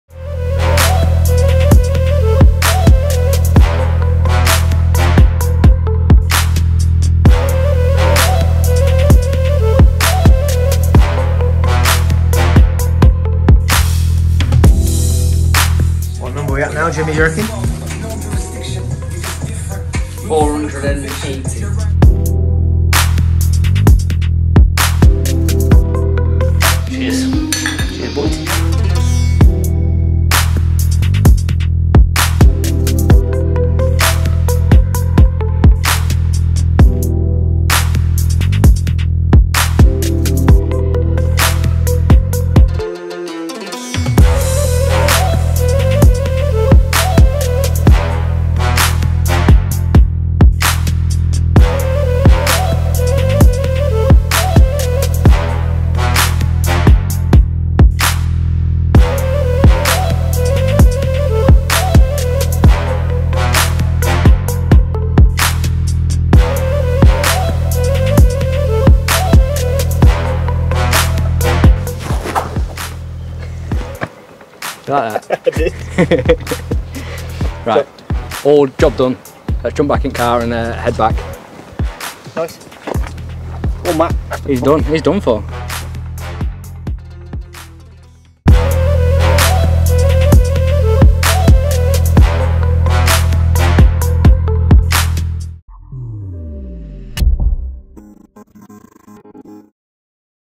500 Espresso Martinis
Our first large commercial order for Glug meant a busy few days but a fun few days none the less!